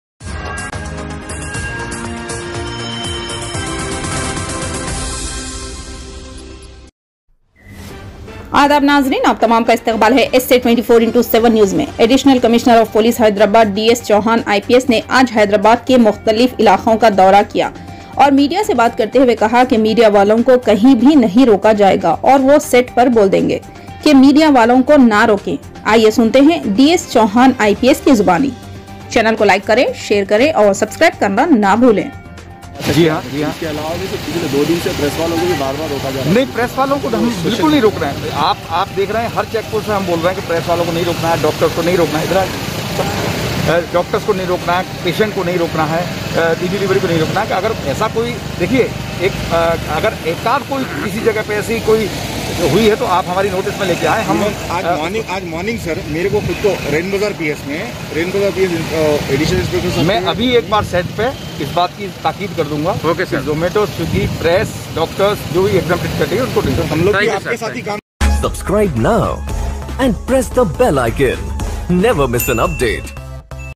Media Walun Ko Kahin Bhi Nahi Roka Jayega Aur Wo Set Par Boldenge Ke Media walon ko Koi bhi Na ruke
Additional commissioner of police Hyderabad
DS Chauhan IPS Ne Aaj  Hyderabad Ke Muqtalif Ilaaqo Ka Daura Kiya Aur Media Se Bhi Baat Karte Hue Kaha Hai Media Walun Ko Kahin Bhi Nahi Roka Jayega Aur Wo Set Par Boldenge Ke Media Walun Ko Na Roke.